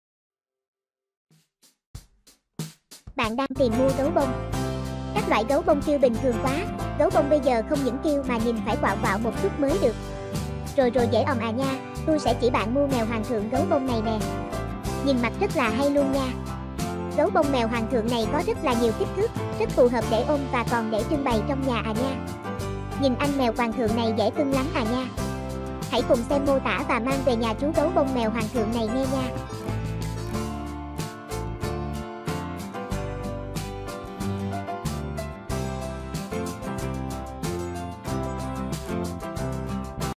Gấu bông mèo hoàng thượng
Mua gấu bông mèo hoàng thượng

Bạn đang tìm mua gấu bông. Các loại gấu bông cute bình thường quá, gấu bông bây giờ không những cute mà nhìn phải quạo quạo một chút mới được. Rồi rồi dễ òm à nha, tui sẽ chỉ bạn mua mèo hoàng thượng gấu bông này nè. Nhìn mặt rất là hay luôn nha. gấu bông mèo hoàng thượng này có rất là nhiều kích thước, rất phù hợp để ôm và còn để trưng bày trong nhà à nha. Nhìn anh mèo quàng thượng này dẽ cưng lắm à nha. Hãy cùng xem mô tả và mang về nhà chú gấu bông mèo hoàng thượng này nghe nha.